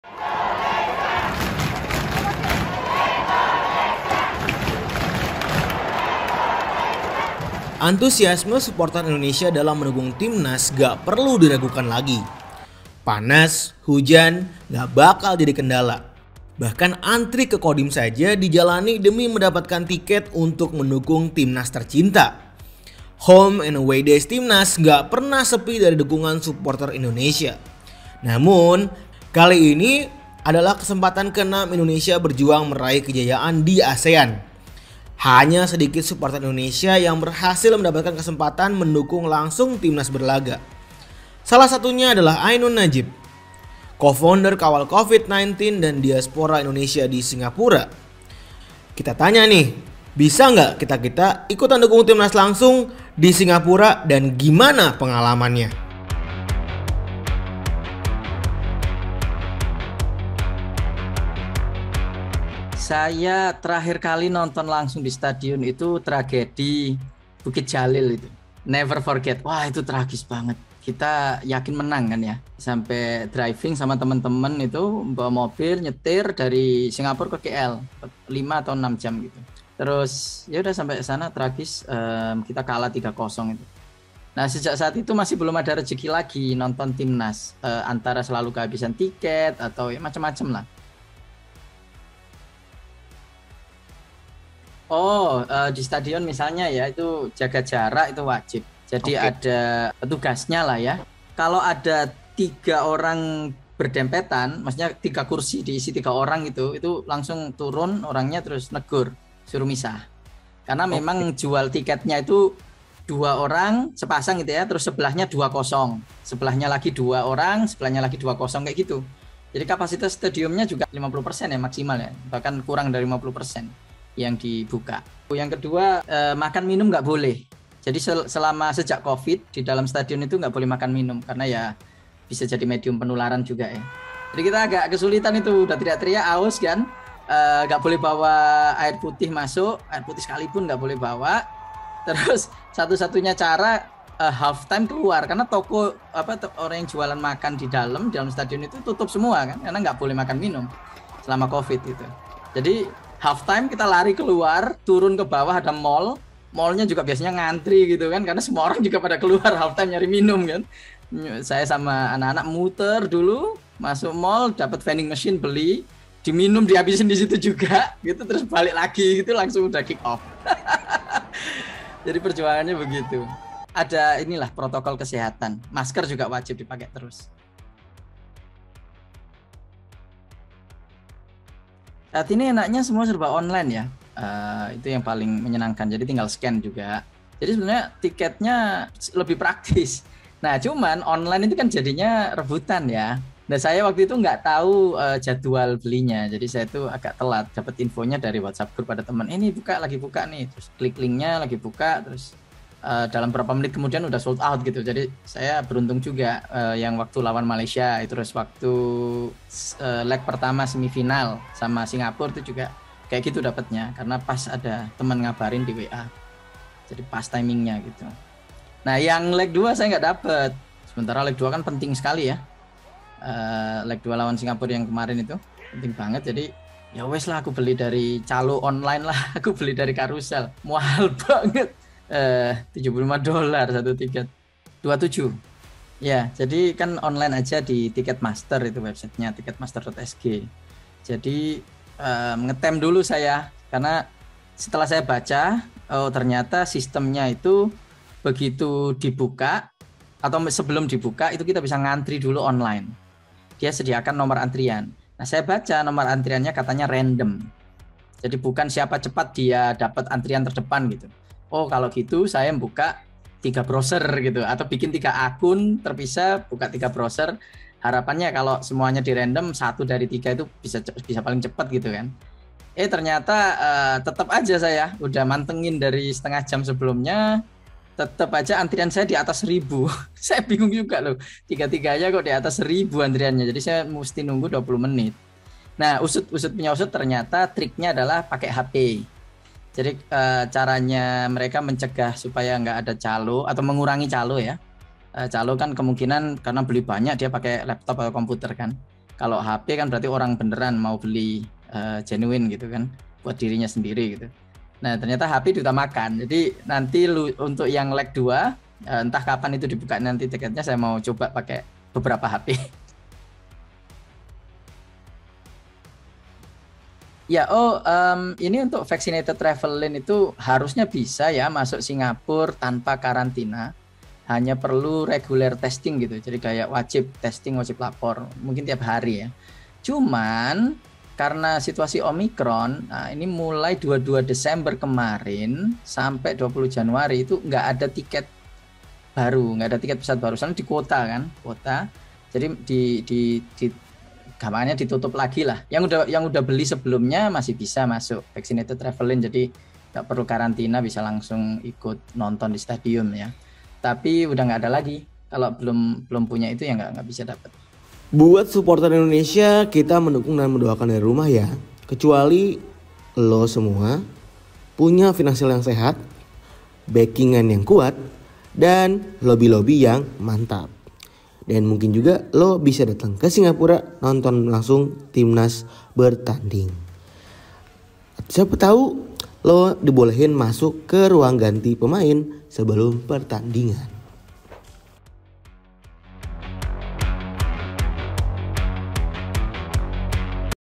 Begini caranya nonton Timnas langsung di Singapura
Antuasiasme suporter Indonesia dalam mendukung Timnas tak perlu diragukan lagi. Panas Hujan nggak bakal jadi kendala. Home and Away Days Timnas tak pernah sepi dari dukungan suporter Indonesia.

Namun kali ini, pandemi Covid-19 menghalangi niat suporter untuk mendukung timnas berlaga di Final Piala AFF 2021.

Hanya sedikit Suporter Indonesia yang berhasil mendapat kesempatan mendukung langsung Timnas Berlaga. Salah satunya adalah Ainun Najib, Relawan Kawal Covid-19 dan Diaspora Indonesia di Singapura.